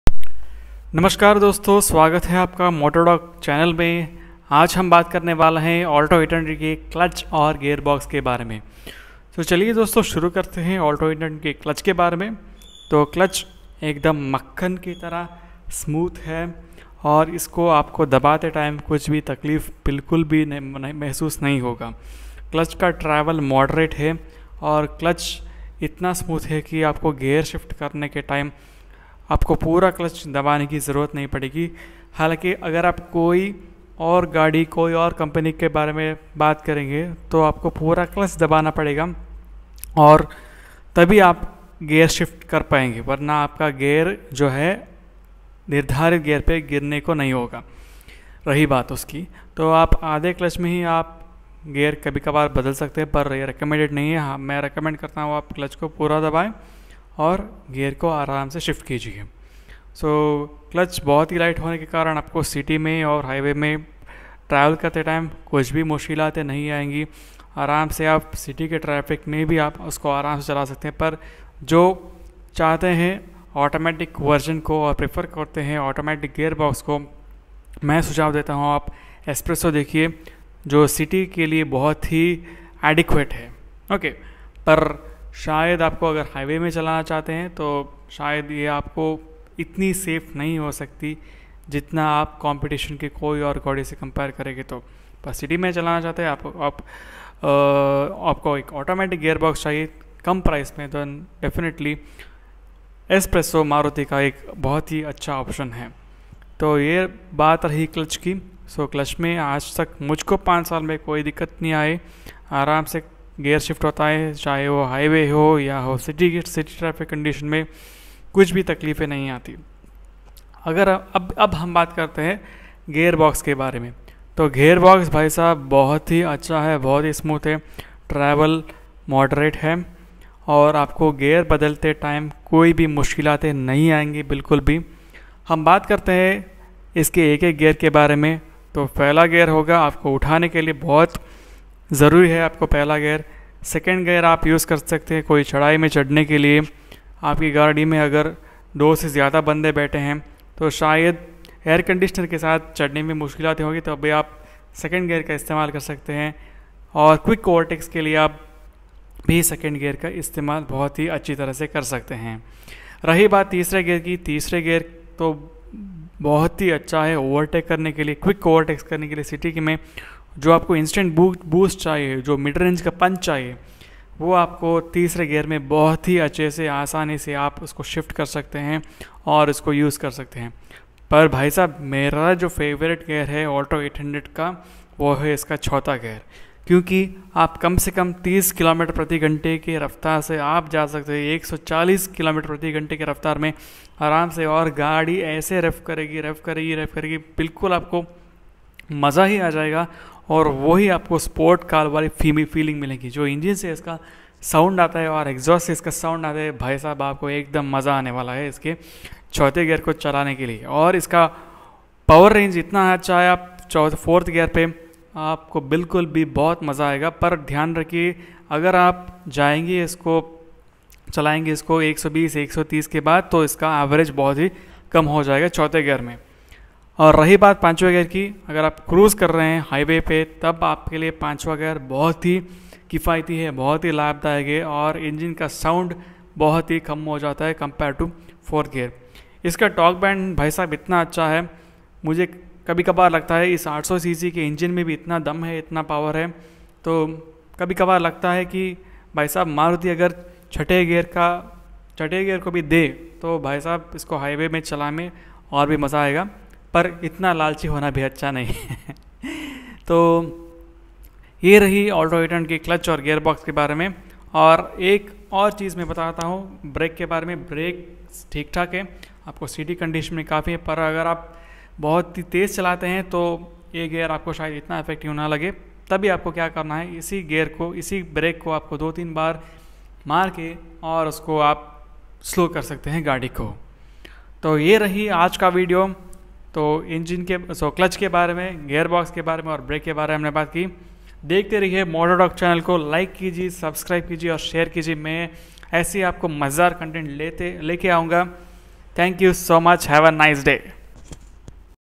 Alto 800 important facts | in hindi | Motor Doc
This video is about 2 most important aspects of alto 800., the clutch and gear box. Additionally braking also discussed

6. This is a review of the vital parts of alto 800, the clutch & gearbox. Also have spoken about my experience of brakes & my personal breaking technique.